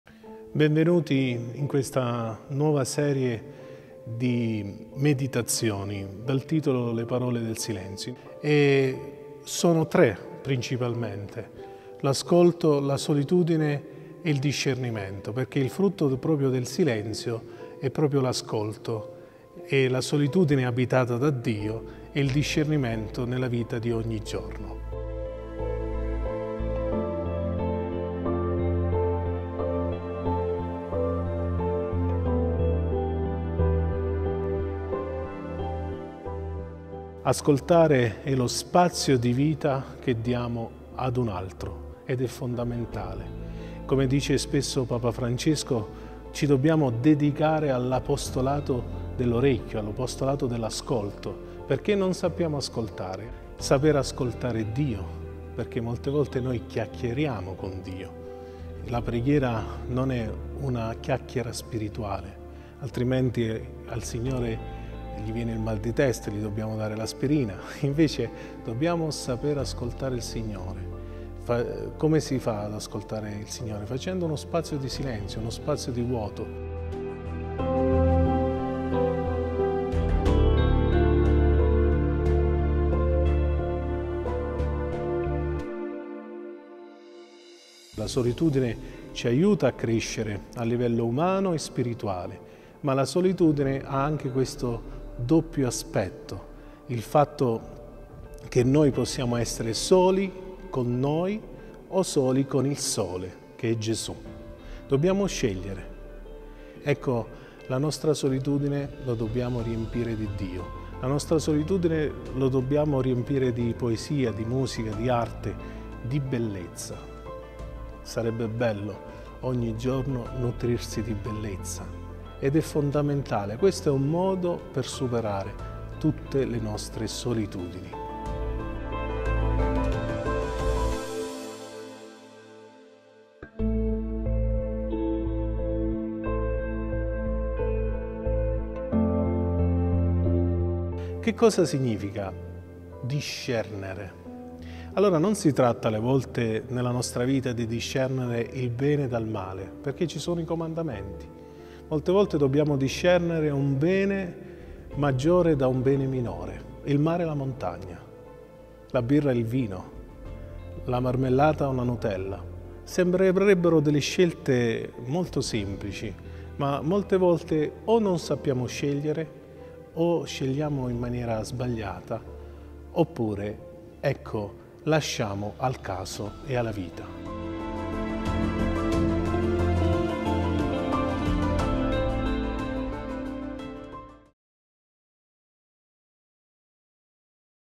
Fra Emiliano Antenucci presenta il ciclo di meditazioni 'Le parole del silenzio'
Un percorso in tre puntate basato sull’esperienza che il francescano ha maturato nell’ambito dei corsi del silenzio, a partire dal 2010. “Uno strumento di ‘cristo-terapia’ per aiutare i fedeli al silenzio interiore”, dice precisando che l’obiettivo è la preghiera.